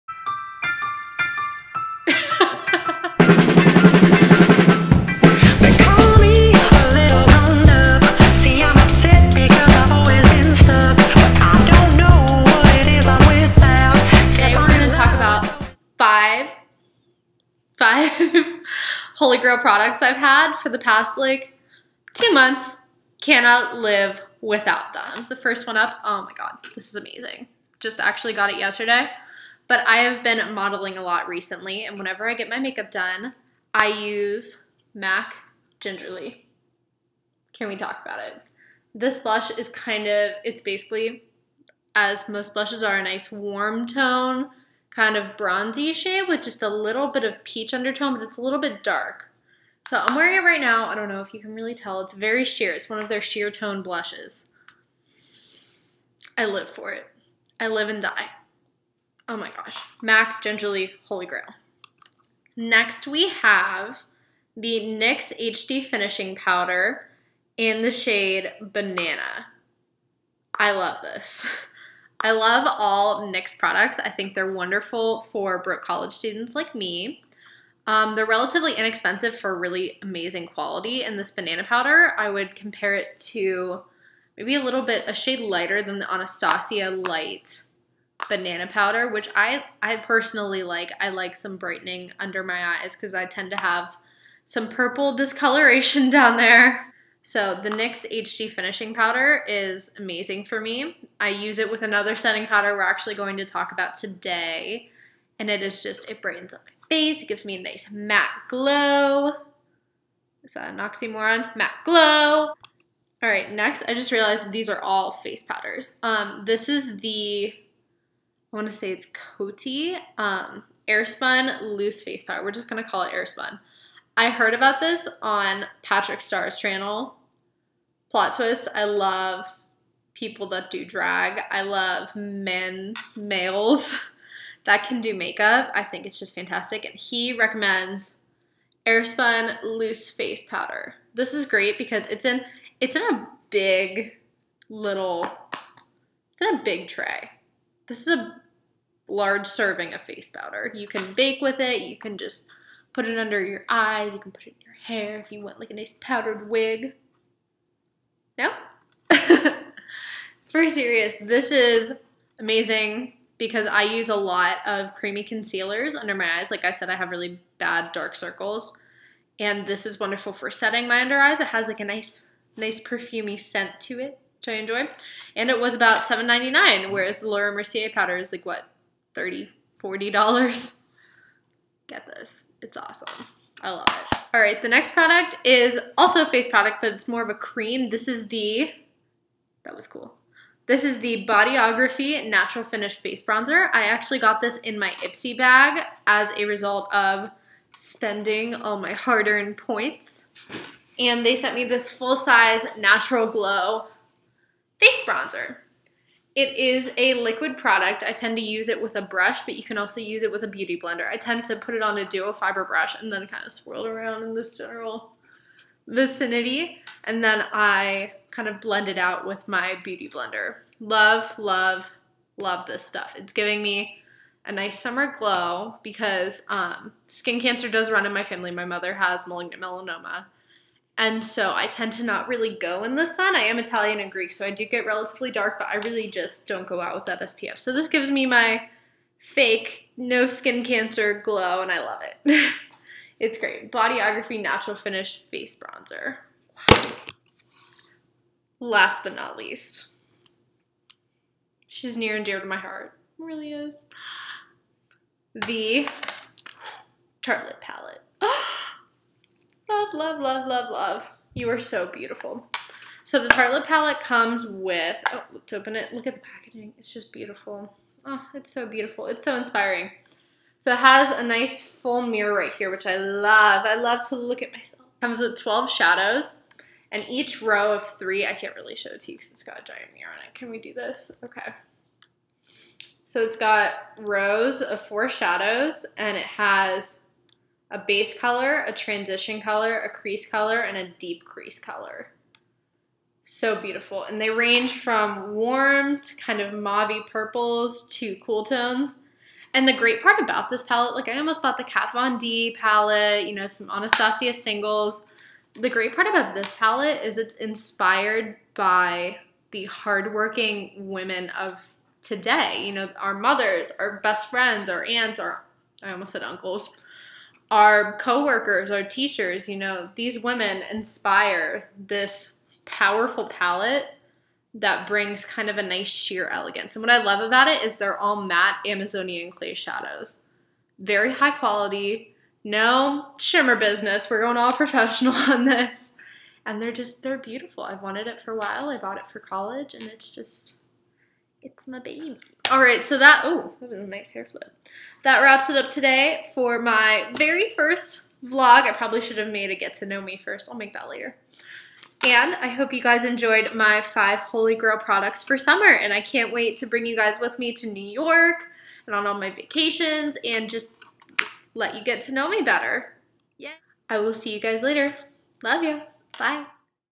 Holy Grail Products
Hi everyone! This is my first vlog/favorites/general talking video! Here are all my favourite products from this summer.